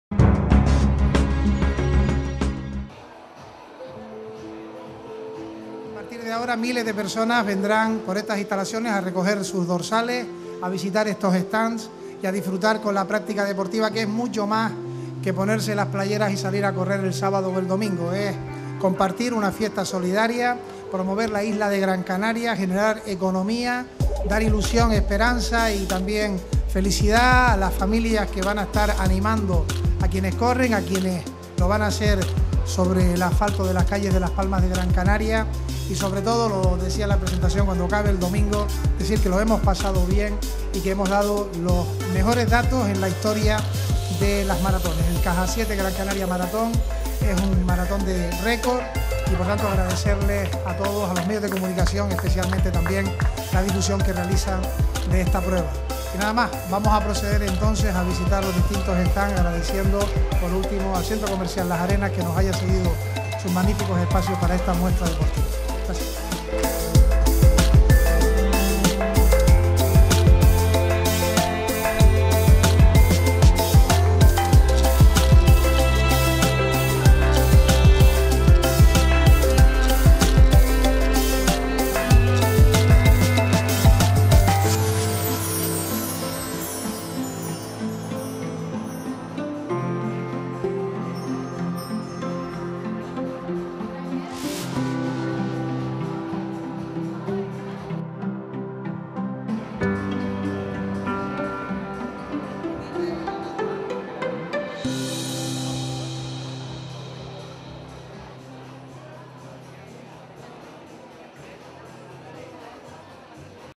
ExpoDeporte, La Feria del Deporte más importante de Canarias arranca con 44 stands
El consejero de Deportes del Cabildo de Gran Canaria, Ángel Víctor Torres, la consejera de  Políticas Sociales y Accesibilidad, Elena Máñez; el gerente de la UTE organizadora del evento, Francisco Armas; el  director de Relaciones Institucionales y Comunicación de Cajasiete, José Manuel Garrido, y el gerente del Centro Comercial Las Arenas, Luis Rivero, han presentado esta mañana  Expodeporte. Esta feria del deporte estará ubicada desde hoy y hasta el sábado 21 de enero, en la planta terraza del Centro Comercial Las Arenas.
Esta Feria, la más importante de Canarias en el ámbito deportivo, aglutina en 44 stands a las empresas, marcas, instituciones, centros  y eventos deportivos que promocionarán sus productos,  la actividad deportiva, las pruebas que organizan y las iniciativas formativas, con el fin de acercar a los ciudadanos el mundo del running.
Enmarcada en el Cajasiete Gran Canaria Maratón, Expodeporte pretende complementar esta importante prueba y aportar un valor añadido atrayendo al público, no solo a la práctica deportiva, sino incitar a aquellos ciudadanos a que participen, haciéndoles partícipes del ambiente deportivo que se genera en torno a los stands.
En este sentido, el consejero de Deportes, Ángel Víctor Torres, quiso agradecer a Cajasiete, a la UTE organizadora del evento, a la Consejería de Políticas Sociales que dirige Elena Máñez y al resto de patrocinadores su “implicación en el que ya es el maratón de los récords, puesto que en esta octava edición cuenta con la participación de 1.400 maratonianos, frente a los cerca de 600 que se inscribieron en la primera edición. Este incremento en el número de atletas, unido al nutrido número de empresas que participan en Expodeporte, demuestra la buena salud del Cajasiete Gran Canaria Maratón.”
Expodeporte podrá visitarse este jueves hasta las 22:00 horas, y viernes y sábado de 10:00 a 22:00 horas.